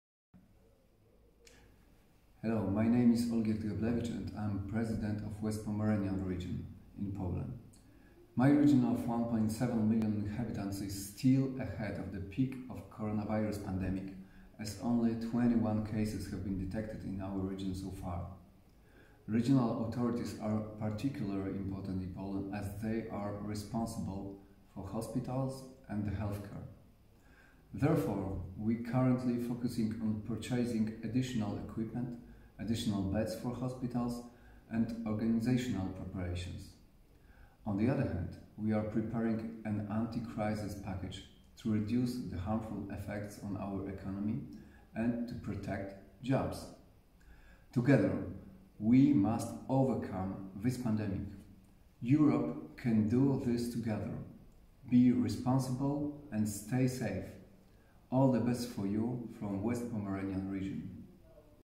Fighting COVID19 at local level - Olgierd Geblewicz, President of the Westpomeranian Region
Olgierd Geblewicz, President of the Westpomeranian Region & President of EPP-CoR Group about fighting COVID19 at local level